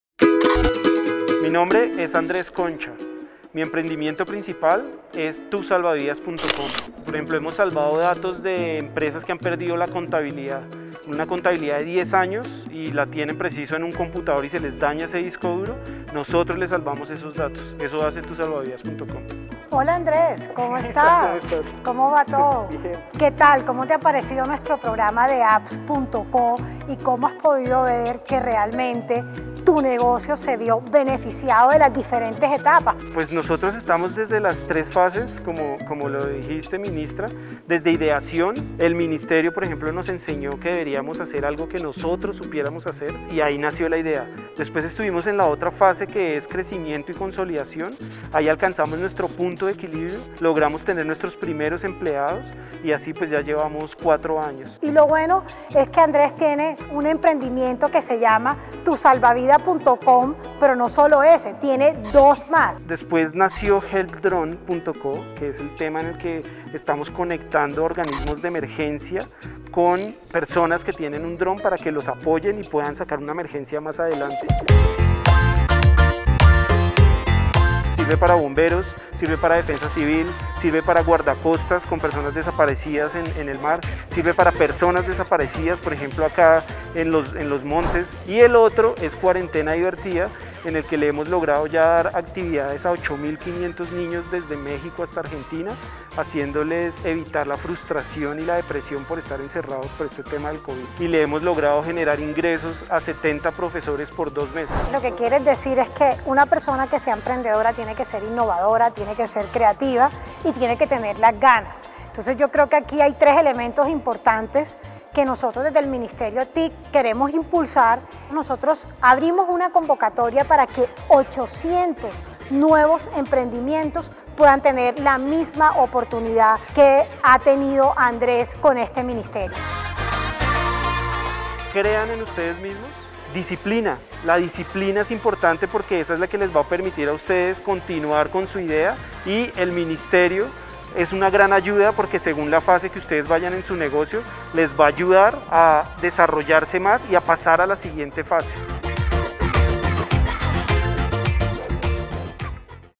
'Tusalvavidas.com', un caso de éxito en las convocatorias de Apps.co
Esta es la historia de un modelo de emprendimiento que dedicó sus esfuerzos por salvar datos perdidos por empresas o personas. Apps.co fue un motor para lograr consolidar esta y otras ideas de negocio.